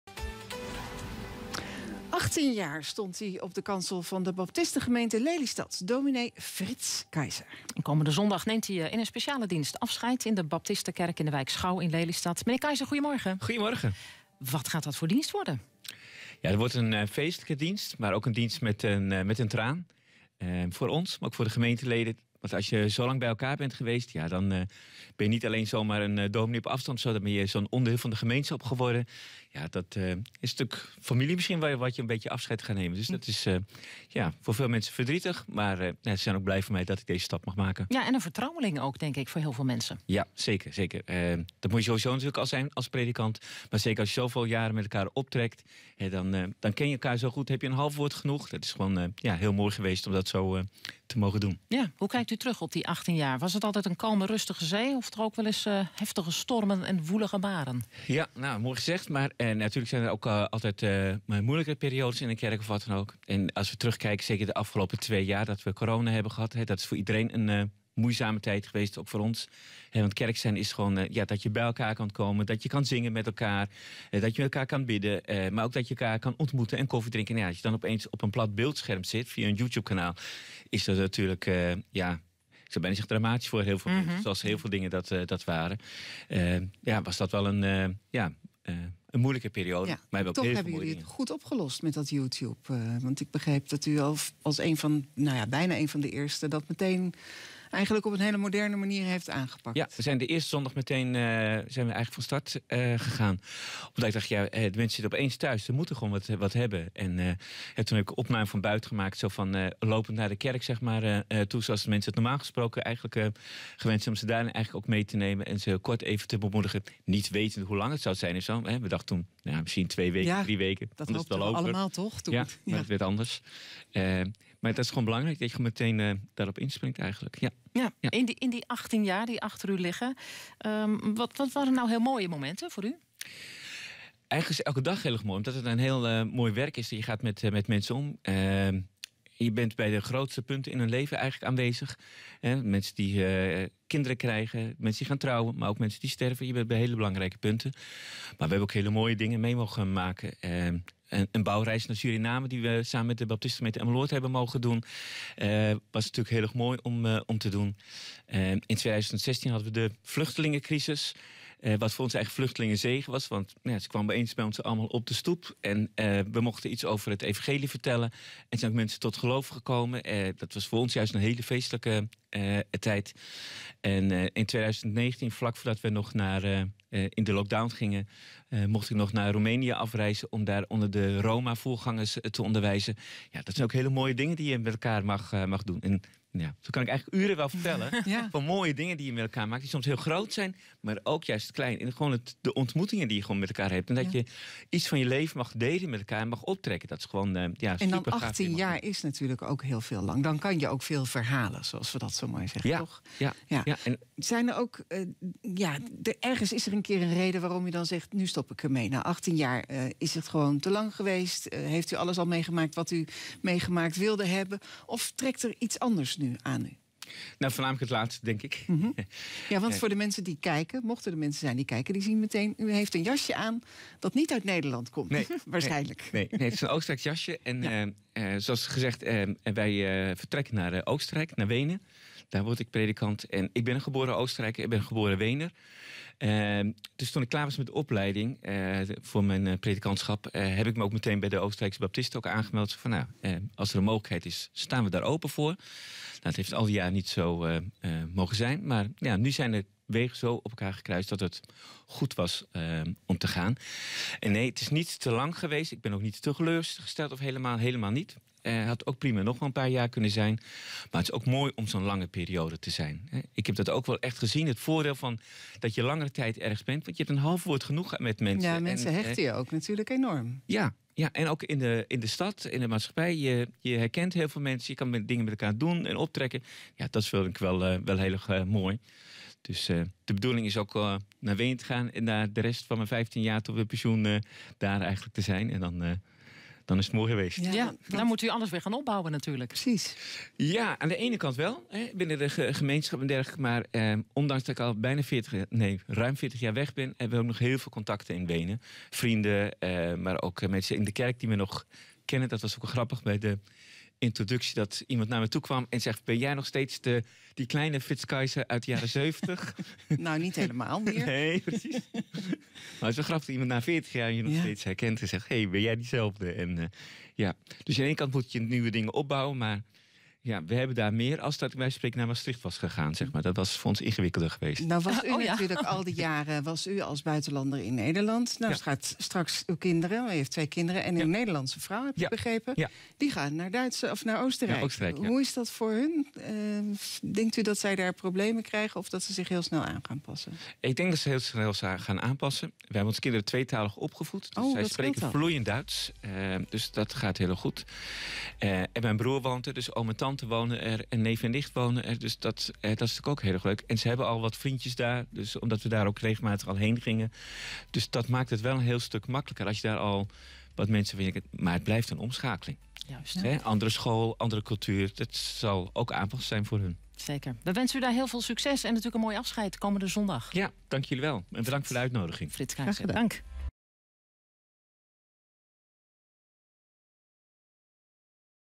Dominee Baptistengemeenschap Lelystad neemt na 18 jaar afscheid
Achttien jaar stond hij op de kansel van de Baptistengemeente Lelystad, dominee Fritz Kaiser. En komende zondag neemt hij in een speciale dienst afscheid in de Baptistenkerk in de wijk Schouw in Lelystad. Kaiser vertrekt naar Oostenrijk.